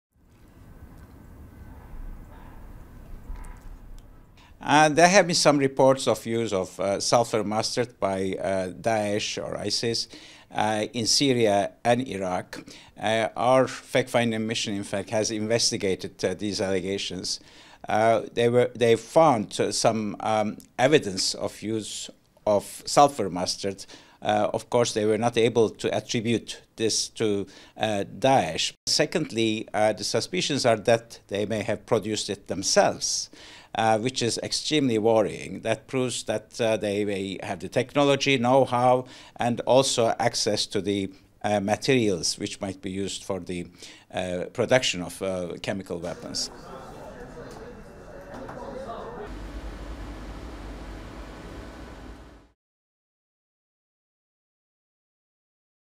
OIAC: l'EI pourrait fabriquer ses propres armes chimiques
Le directeur général de l'OIAC a assuré mardi 3 mai qu'il y a des signes permettant de penser que l'organisation Etat islamique (EI) est en état de fabriquer ses propres armes chimiques en Syrie et en Irak, signes qu'il qualifie d'"extrêmement inquiétants". Durée: 00:56